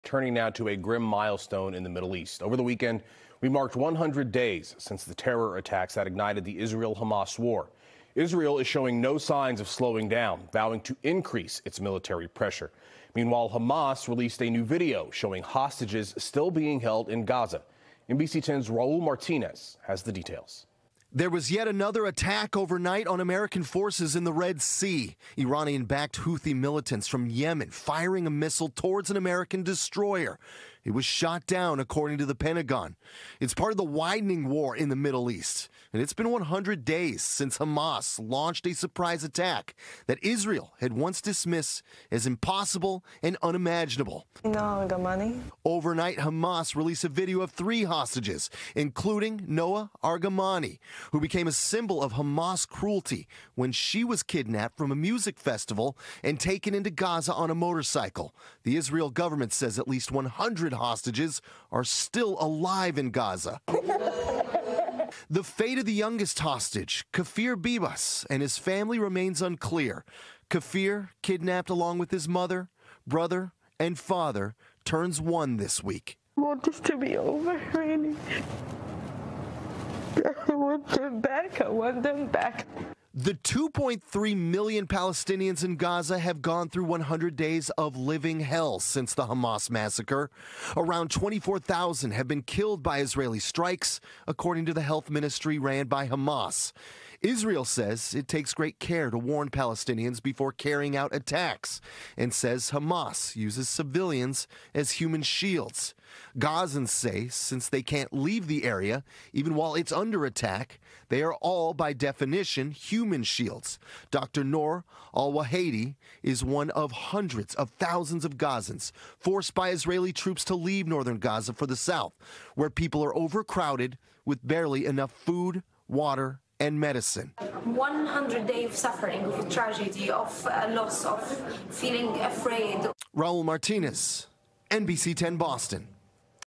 100 days of fighting between Israel and Hamas
It's a grim milestone in the Middle East. It's been 100 days since the terror attack that ignited the Israel-Hamas war. Israel is showing no signs of slowing down, vowing to increase military pressure. Meanwhile, Hamas released a new video showing hostages still being held in Gaza.